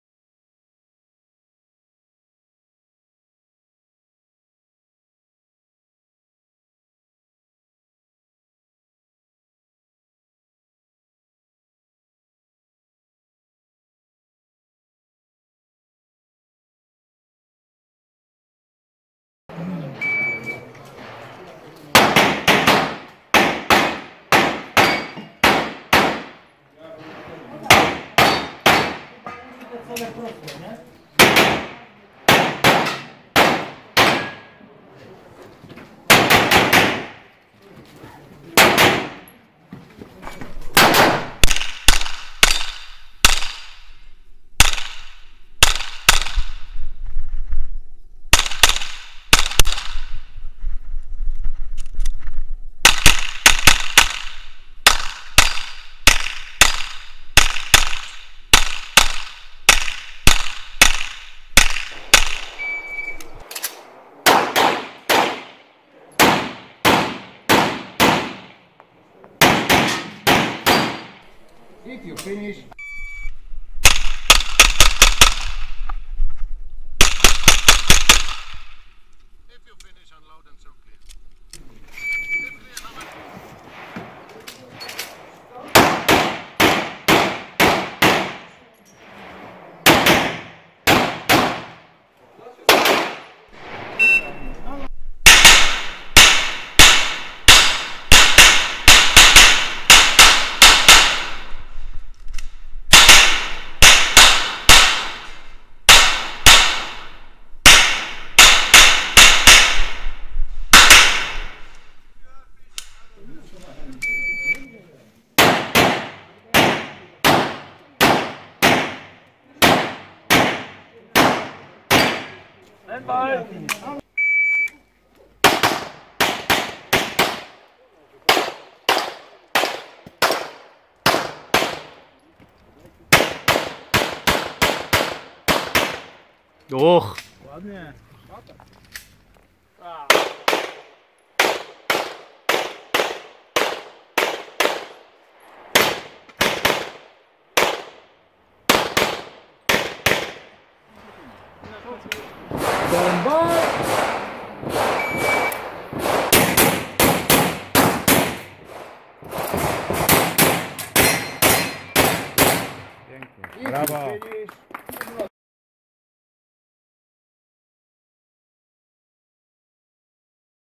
IPSC LV. III Incorsa Cup 2017 Owiess
Pierwsze rankingowe zawody w tym sezonie i pierwsze moje w klasie Standard. Udało się rozpocząć sezon z przytupem zajmując 3. miejsce. Standard to nie jedyna zmiana, powstał Shooting Team Good Fellas powered by Tanfoglio, którego mam zaszczyt być członkiem :) Na ten temat wkrótce więcej informacji na facebooku ;)